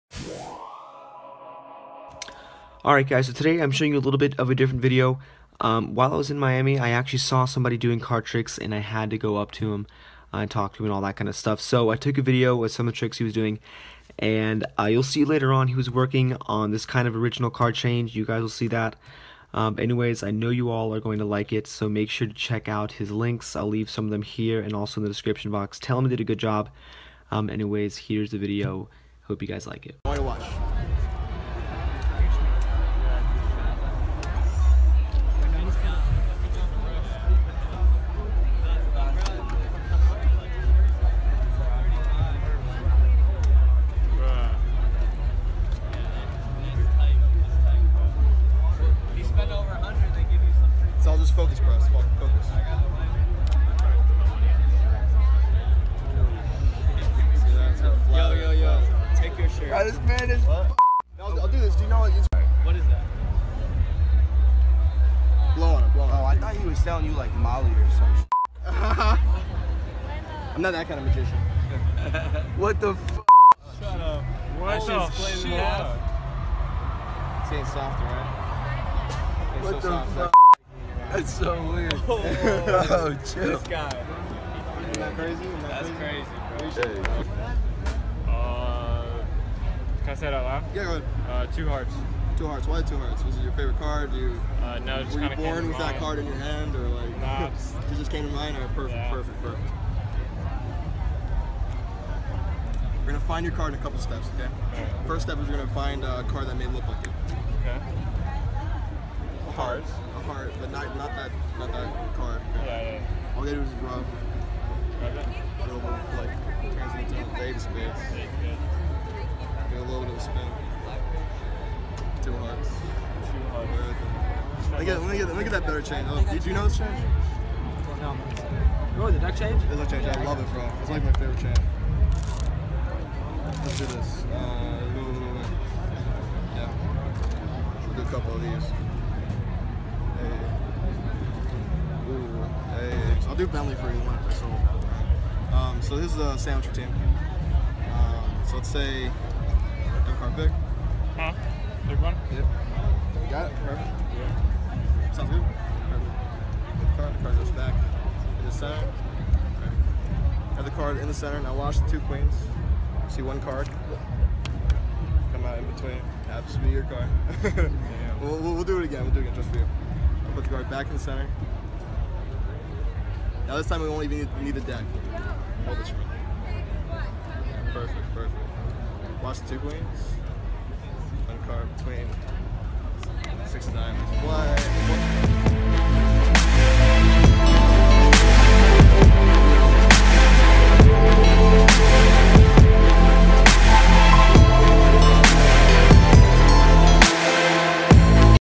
Better Than David Blaine Street Magic Performance!!
Alright guys, so today I'm showing you a really cool street magic performance that I saw while at Rolling Loud in Miami! Out of the corner of my eye I see a guy doing card tricks to strangers so I had to see for myself. Turns out he was really good, make sure to check him out!!